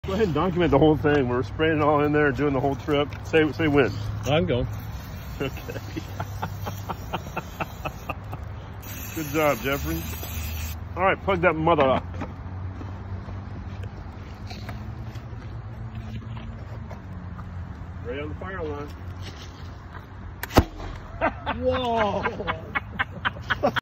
Leopold Gallery Staff Shooting a Potato Gun
The next shot went even farther than this one. Someone needs to grow up.